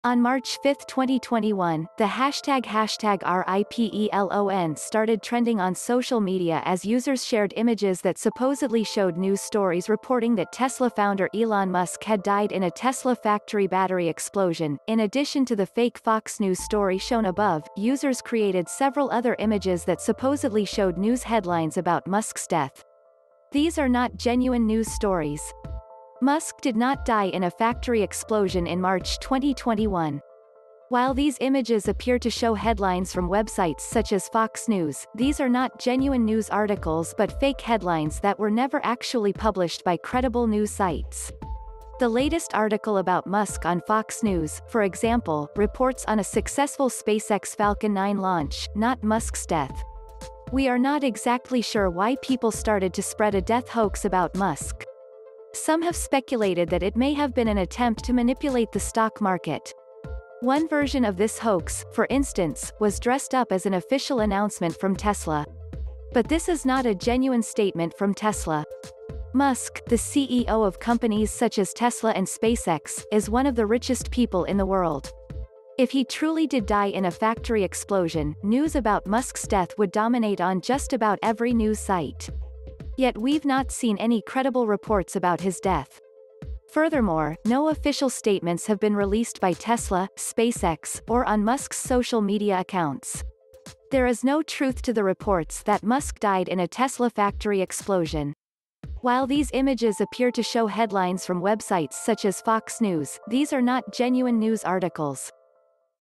No Elon Musk Did Not Die in a Car Battery Explosion
On March 5, 2021, the hashtag RIPELON started trending on social media as users shared images that supposedly showed news stories reporting that Tesla founder Elon Musk had died in a Tesla factory battery explosion:In addition to the fake Fox News story shown above, users created several other images that supposedly showed news headlines about Musk’s death. These are not genuine news stories. Musk did not die in a factory explosion in March 2021. While these images appear to show headlines from websites such as Fox News, these are not genuine news articles but fake headlines that were never actually published by credible news sites. The latest article about Musk on Fox News, for example, reports on a successful SpaceX Falcon 9 launch, not Musk’s death. We are not exactly sure why people started to spread a death hoax about Musk. Some have speculated that it may have been an attempt to manipulate the stock market. One version of this hoax, for instance, was dressed up as an official announcement from Tesla. But this is not a genuine statement from Tesla. Musk, the CEO of companies such as Tesla and SpaceX, is one of the richest people in the world. If he truly did die in a factory explosion, news about Musk’s death would dominate on just about every news site. Yet we’ve not seen any credible reports about his death. Furthermore, no official statements have been released by Tesla, SpaceX, or on Musk’s social media accounts. There is no truth to the reports that Musk died in a Tesla factory explosion. While these images appear to show headlines from websites such as Fox News, these are not genuine news articles.